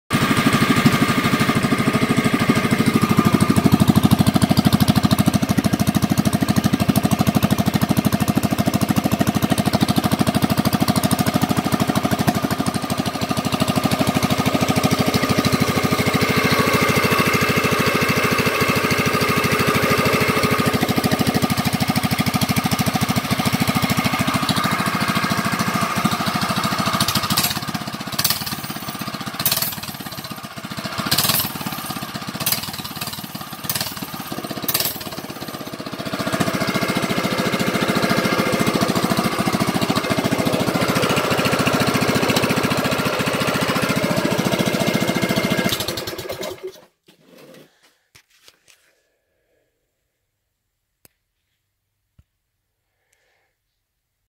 Predator 212cc exhaust header made from a chair.
Same as title.  I wanted a header but didn't want to pay $30 from Go power sports.